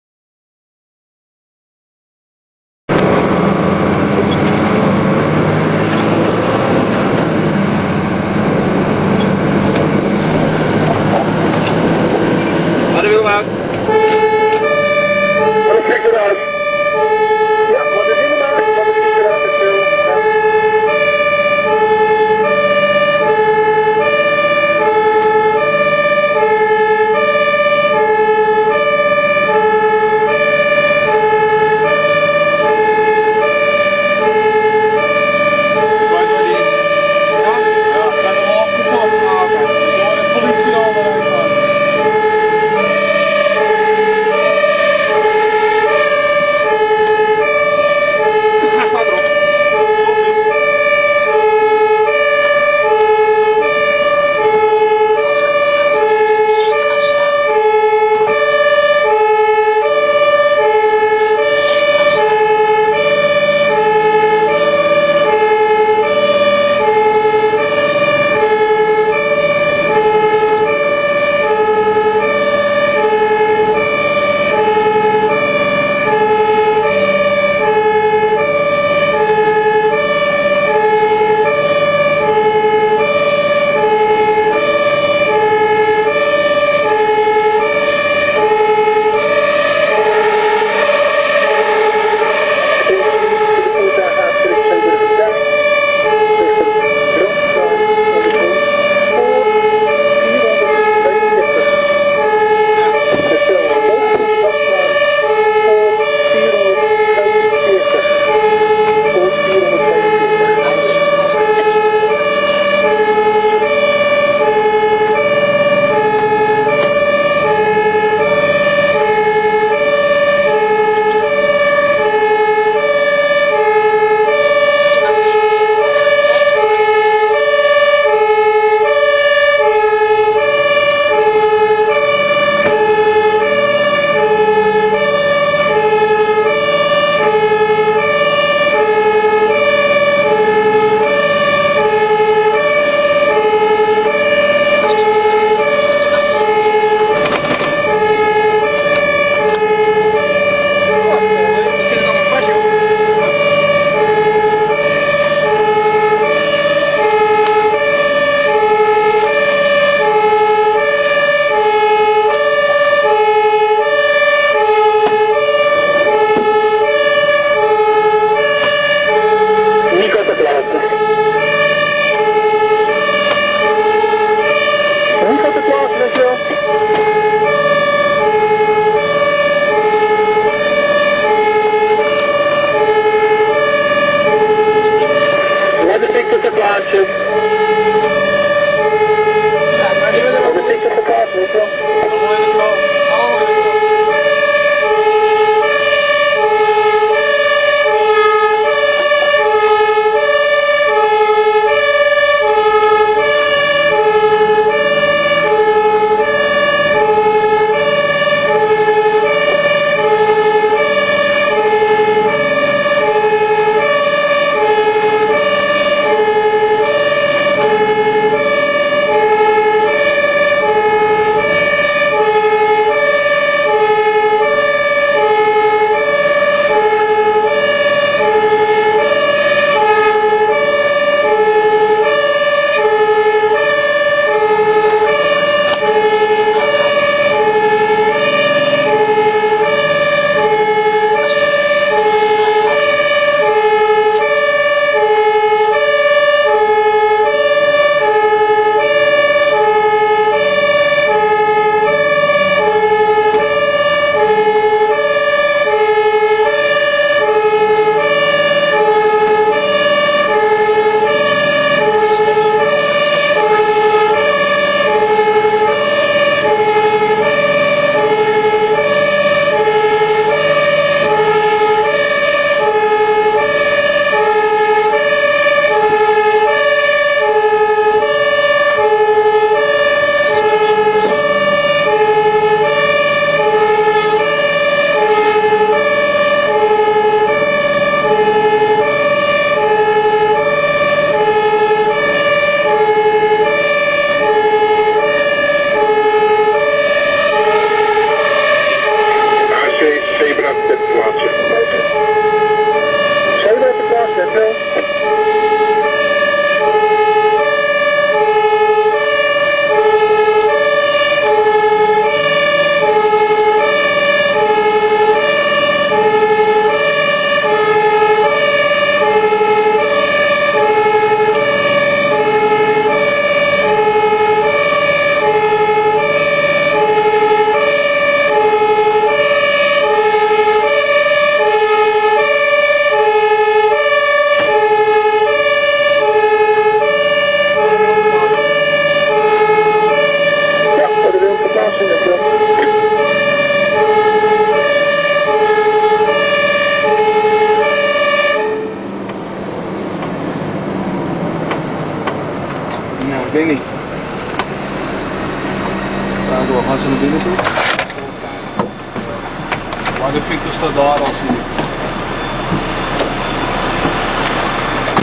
Brand Zeeburgerdijk - Ladder Willem (2007)
What: Housefire
Where: Zeeburgerdijk in Amsterdam
Filmed on: Ladder Willem 558

Wat: Brand in een woning
Waar: Zeeburgerdijk te Amsterdam
Gefilmd vanuit: Ladder Willem 558